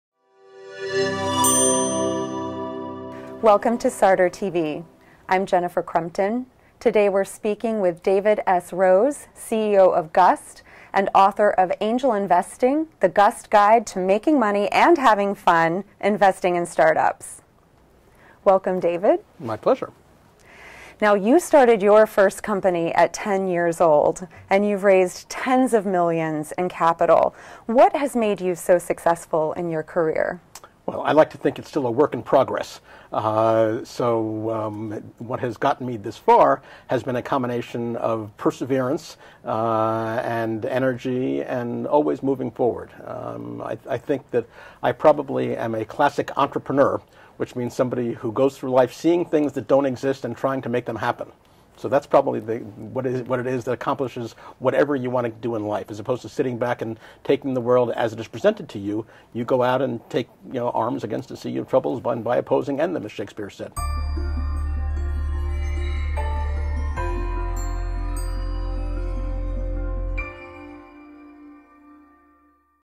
What has made you successful in your career? by David S. Rose, Author of Angel Investing
David S. Rose has been described as "New York's Archangel" by Forbes, "patriarch of Silicon Alley" by Red Herring, and a "world conquering entrepreneur" by BusinessWeek. He is a serial entrepreneur, an Inc 500 CEO, and one of the world's most active angel investors. David is CEO of Gust, which operates the standard collaboration platform for over 300,000 startups and 50,000 early-stage investors, and is the author of the New York Times best selling book "Angel Investing: The Gust Guide to Making Money & Having Fun Investing in Startups".

He founded and is Chairman Emeritus of New York Angels and is on the boards of KoolSpan, Waywire, Social Bicycles, Miner and TekServe. Through his early stage technology venture capital firm Rose Tech Ventures, he has helped to fund over 110 early stage companies, with exits to companies including Facebook, Google, Intel, Amazon, and other industry leaders.

David founded the Finance, Entrepreneurship & Economics track at Singularity University, and is a keynote speaker on the future of business and technology. Dubbed by BusinessWeek "The Pitch Coach" as an advisor to entrepreneurs perfecting their fundraising skills, he is a frequent guest lecturer at business schools including Yale, Harvard, Columbia, NYU and Venture for America. David founded wireless Internet pioneer AirMedia in 1988, and in 1997 was honored as a finalist for Ernst & Young's Entrepreneur of the Year Award.

Earlier in his career David was co-founder of one of the world’s first computer training firms, and spent a decade developing technology-enhanced real estate. Prior to that, he served as Deputy Director of the New York office of US Senator Daniel Patrick Moynihan.

A native New Yorker, David has a B.A. from Yale (where he is an Associate Fellow of Pierson College), an M.B.A. in Finance from Columbia Business School and a D.Eng. (hc) from Stevens Institute of Technology. He is a graduate of the New York City public school system and Horace Mann High School.

David Rose, CEO of Gust, successful serial entrepreneur, and author of, Angel Investing: The Gust Guide to Making Money & Having Fun Investing in Startups, talks with Jennifer Crumpton, SarderTV correspondent, about what has led to his professional success.